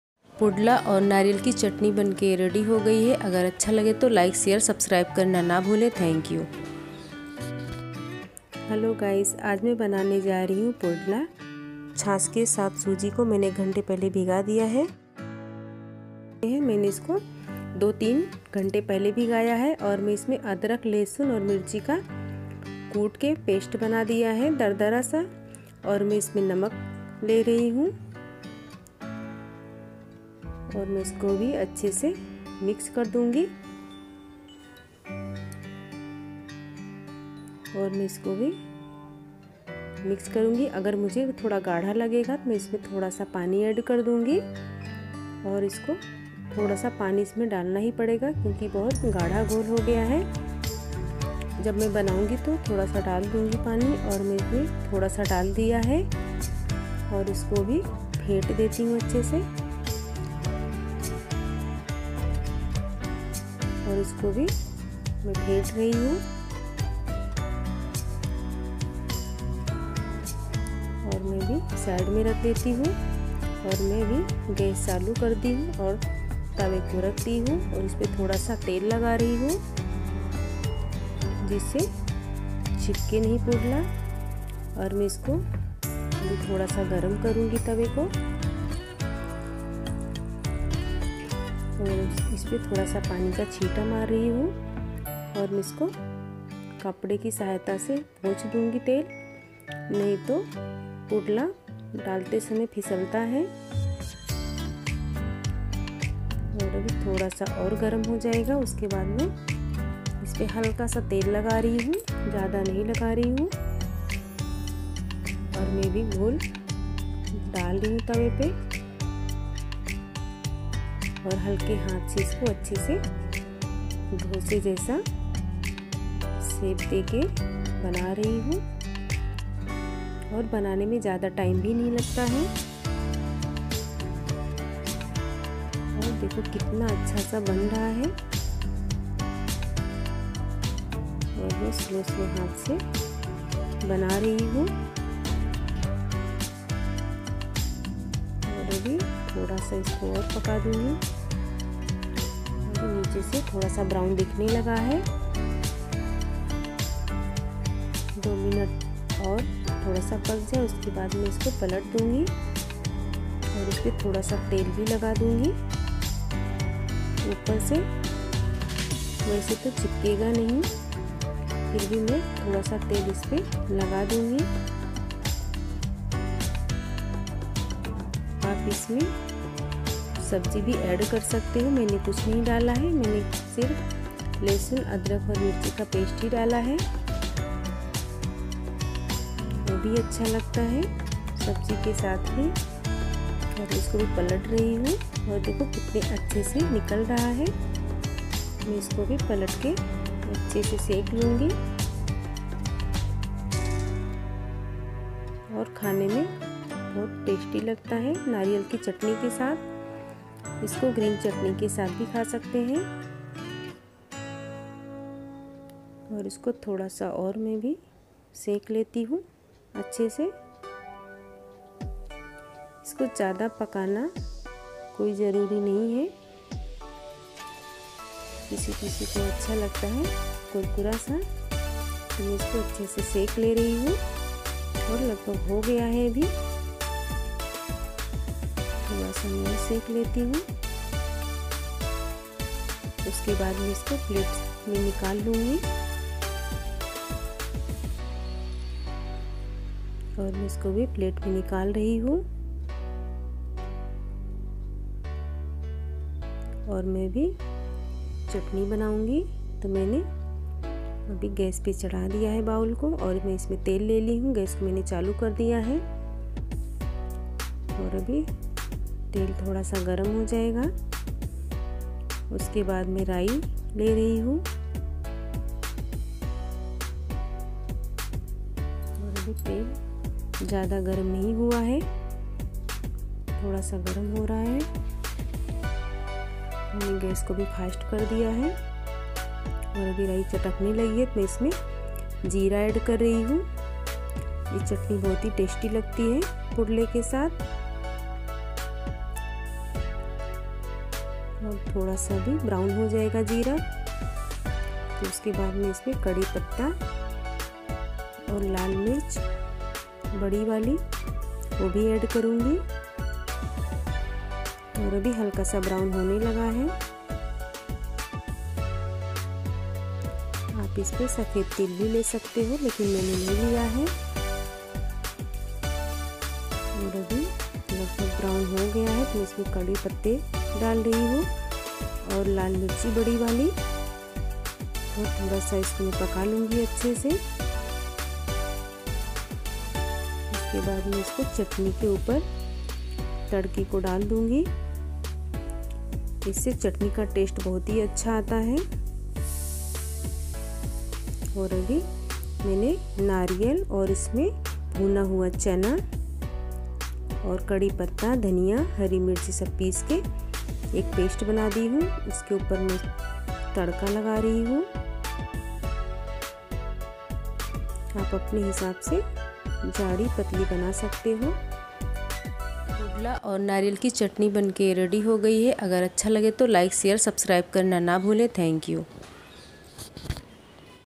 बिना तले बनाए पुडला और नारियल की चटनी | Poonam Kitchens
टेस्टी पुडला और नारियल की चटनी की रेसिपी | Poonam Kitchens

Ingredients:-
Rava
Buttermilk
Adrak lehsun mirchi ka paste
Namak
COCONUT
RAI
LAL MIRCH
CURRY LEAVES